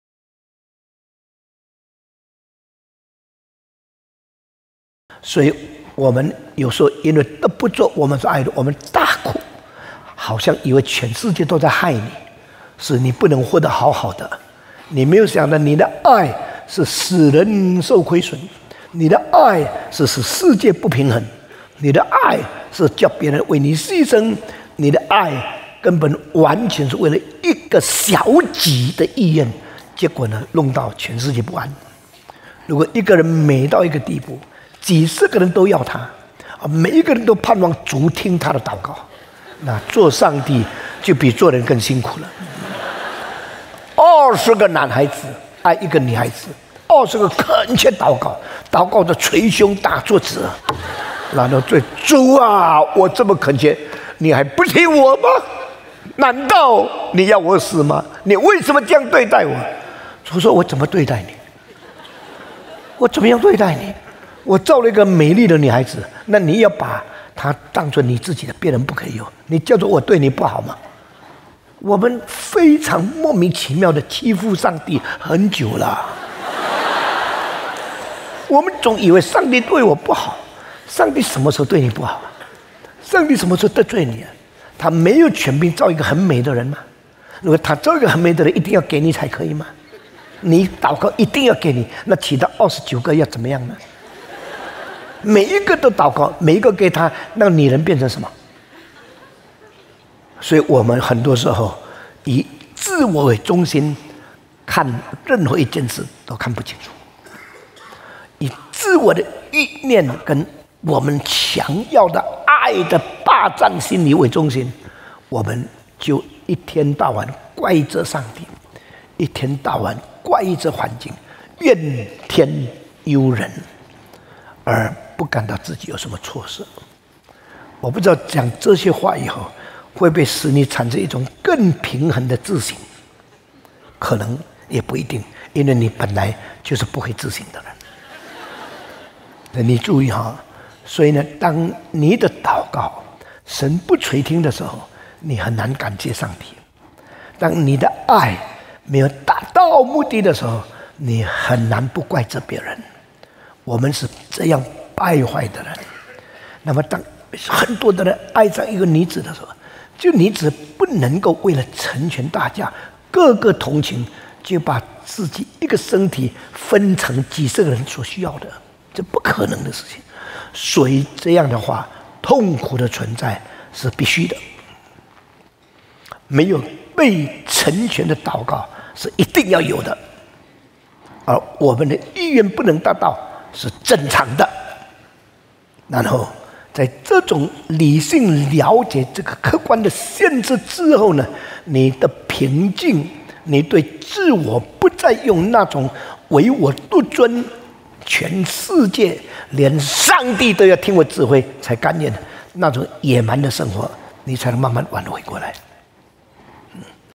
得不到所愛，就怪上帝嗎？（婚前愛與性要理問答48問）
所以我們有時候因為得不著我們所愛的而大哭，好像以為全世界都在害你，使你不能活得好好的，你沒有想到你的愛是使人受虧損、你的愛是使世界不平衡、你的愛是叫別人為你犧牲、你的愛根本完全是為了一個小己的意願，結果弄到全世界不安。

如果一個人美到一個地步，幾十個人都要他，每一個人都盼望主聽他的禱告，那作上帝就比作人更辛苦了。廿個男孩子愛一個女孩子，廿個懇切禱告，禱告的時候捶胸打桌子說：「主啊，我這麼懇切，祢還不聽我？難道祢要我死嗎？祢為什麼這樣對待我？」主說：「我怎麼對待你？我要怎麼樣對待你？我造一個美麗的女孩子，你要把他當作你自己的，別人不可以有，這叫做我對你不好嗎？」我們非常莫名其妙的欺負上帝很久了。

我們總以為上帝對我不好，上帝什麼時候對你不好？上帝什麼時候得罪你？祂沒有權柄造一個很美的人嗎？如果祂造一個很美的人，一定要給你才可以嗎？你禱告一定要給你，那其他二十九個要怎麼樣呢？每一個都禱告，每一個都給他，那個女人變成什麼？

所以我們很多時候以自我為中心，看任何一件事都看不清楚；以自我的慾念跟所強要的愛的霸佔心理為中心，我們就一天到晚怪責上帝、一天到晚怪責環境，怨天尤人，而不感到自己有什麼錯失。

我不知道講這些話以後，會不會使你產生一種更平衡的自省。可能也不一定，因為你本來就是不會自省的人。

你注意啊！所以呢，當你的禱告，神不垂聽的時候你很難感謝上帝。當你的愛、沒有達到目的的時候，你很難不怪責別人，我們是這樣敗壞的人。

當很多的人愛上一個女子的時候，這女子不能夠為了成全大家、個個同情，就把自己一個身體分成幾十個人所需要的，這不可能的事情。所以這樣的話，痛苦的存在是必須的，沒有被成全的禱告，是一定要有的，而我們的意願不能達到，是正常的。

然後在這種理性瞭解這個客觀的限制之後，你的平靜，你對自我不再用那種唯我獨尊、全世界連上帝都要聽我指揮才甘願的那種野蠻的生活，你才慢慢挽回過來。